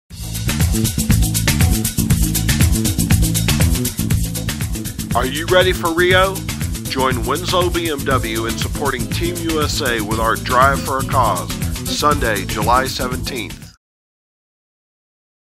Drive for Team USA Register today
Drive for a Cause, supporting Team USA. Join us on Sunday, July 17, 2016 between 11:00am - 6:00 pm.

I-25 @ N. Nevada Avenue, Exit 148

With each test drive, a donation will be made to Team USA. Plus, you will receive $1,000 certificate towards a new car purchase.